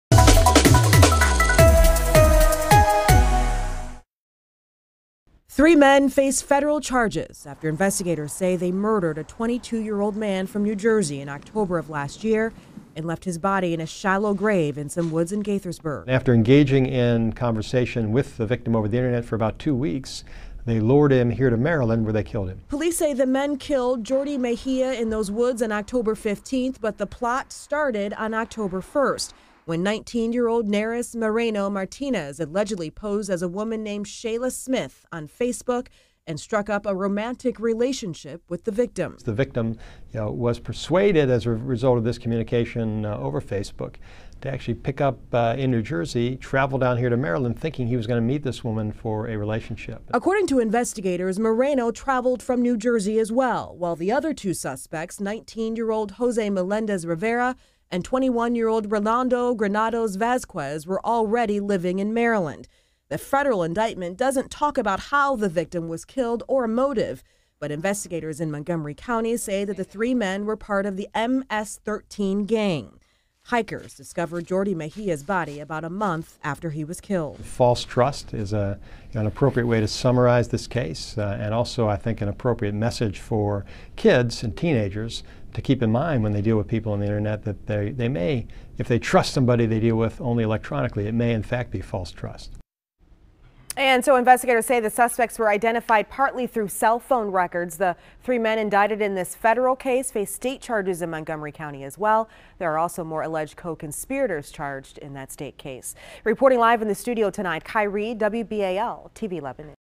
Video: Men used social media to lure, kill victim, prosecutors say
A federal grand jury has indicted three men and accused them of using Facebook to lure a man from New Jersey to Maryland to kill him. The homicide occurred five months ago and police said the suspects buried the victim's body in a shallow grave in Gaithersburg.